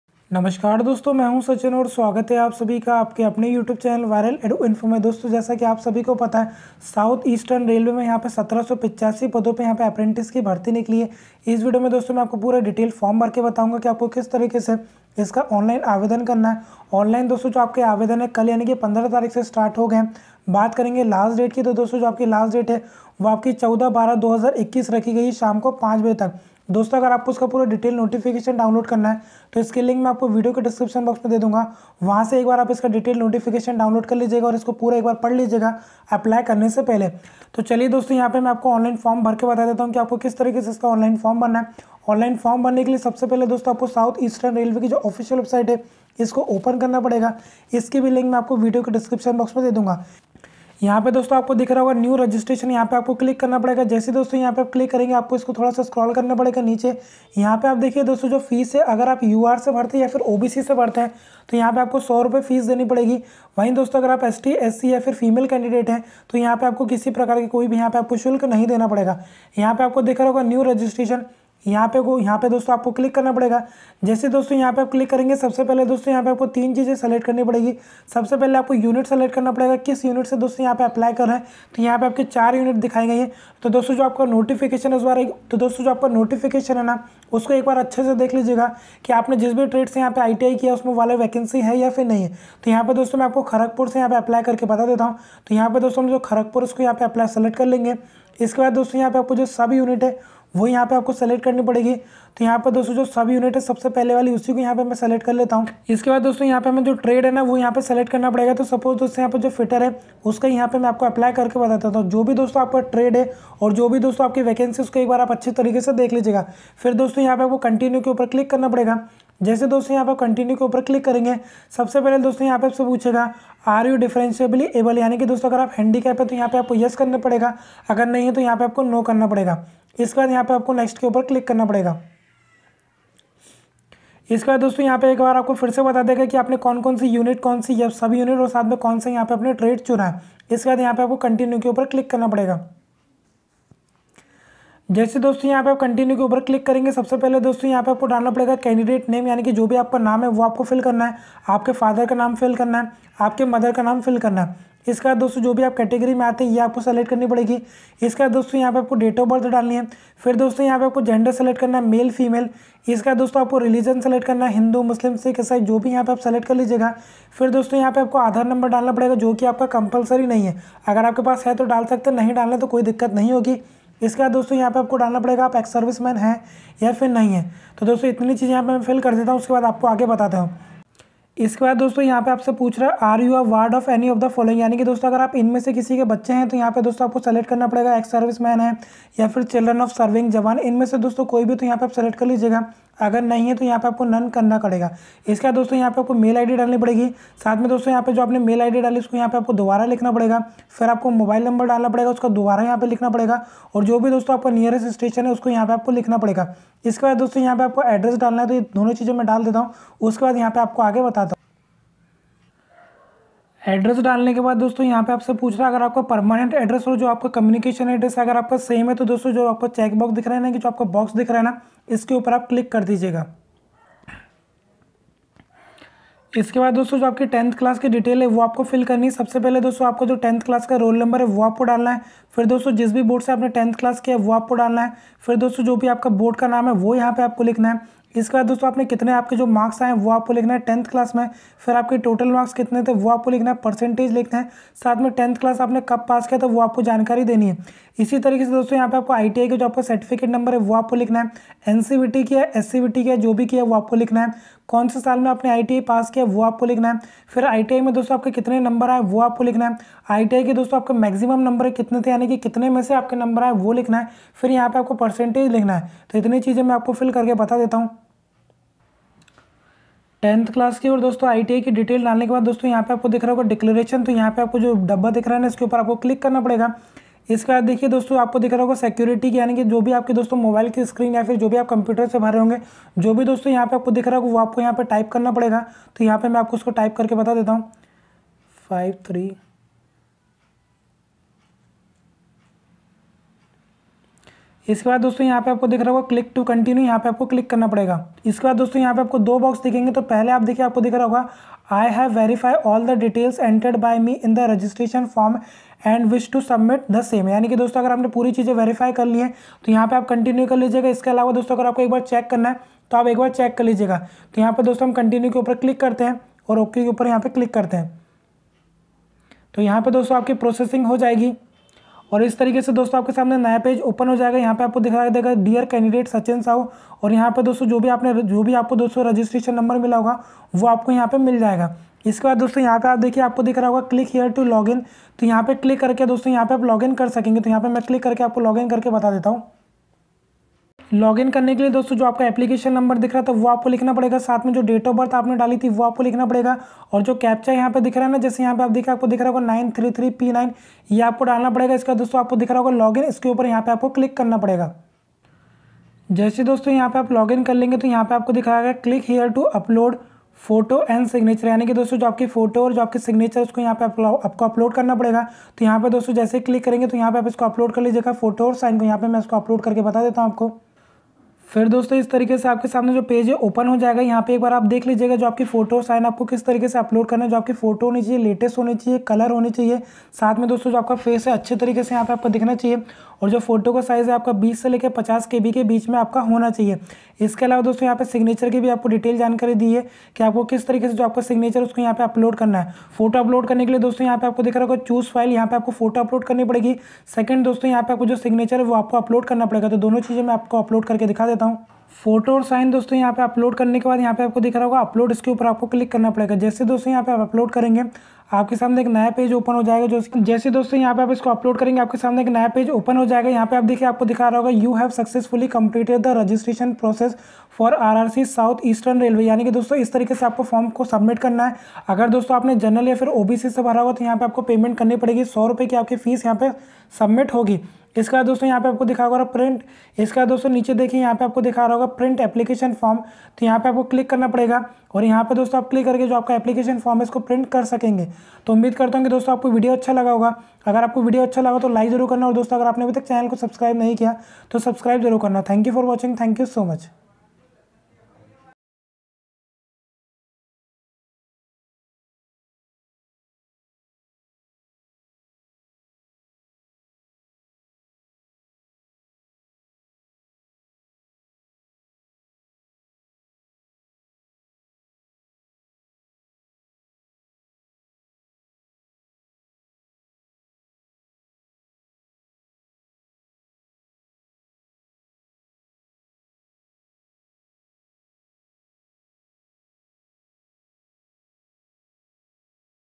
South Eastern Railway Apprentices 2021 online form kaise bharen | How to fill SER Apprentice form
SER Apprentice Form 2021 Video Timeline:

00:00 Railway SER Apprentice Online Form 2021 notification
02:00 How to Fill Railway SER Apprentice Form 2021
04:19 RRC SER Apprentice Online Form 2021
05:10 SER Apprentices form photo sign upload
06:23 RRC SER Apprentice Online Form 2021 Payment
07:14 RRC SER Apprentice Online Form 2021 Print out